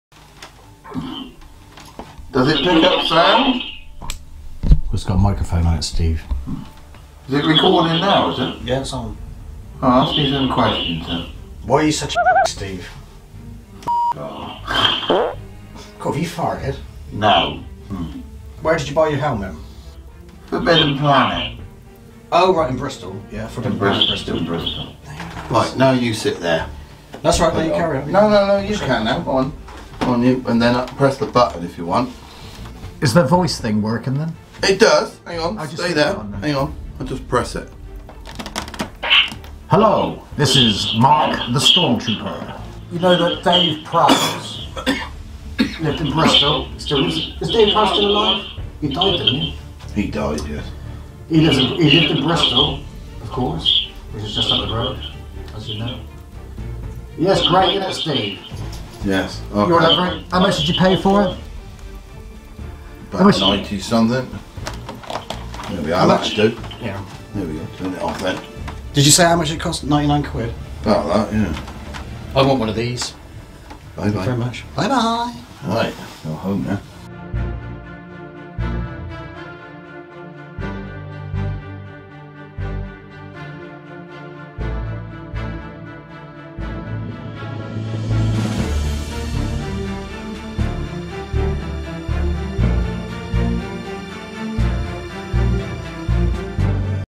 STAR WARS - THE LAST JEDI | Hasbro Black Series Electronic First Order STORMTROOPER HELMET
A quick look at the First Order Stormtrooper Helmet from Star Wars: The Last Jedi by Hasbro.

Star Wars The Black Series First Order Stormtrooper Premium Electronic Helmet, The Last Jedi Roleplay Collectible

Amazon Storefront: